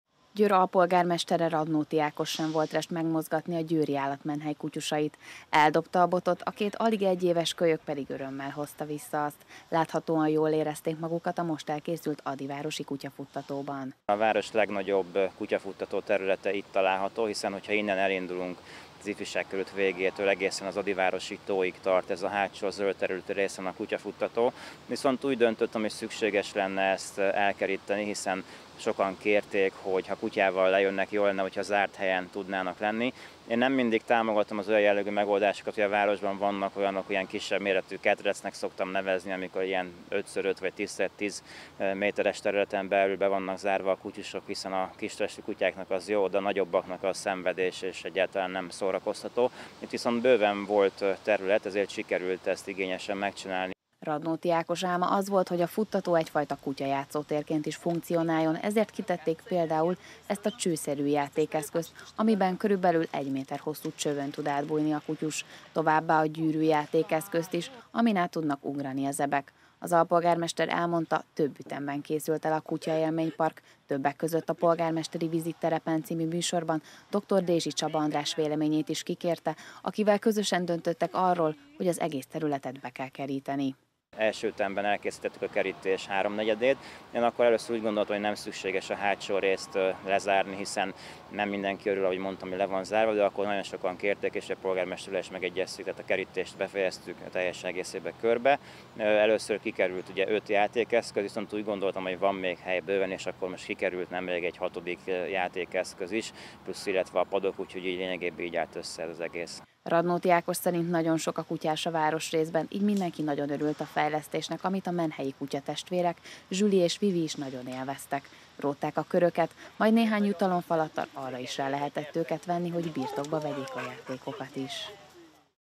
Elkészült az adyvárosi kutyafuttató
Mostanra már mindenhol zárt kerítés és hat kutyajáték is várja a mozgékony ebeket és a gazdikat az adyvárosi kutyafuttatóban. Radnóti Ákos, alpolgármester ma mutatta be a Győrplusznak az elkészült futtatót, amely több ütemben nyerte el végleges formáját.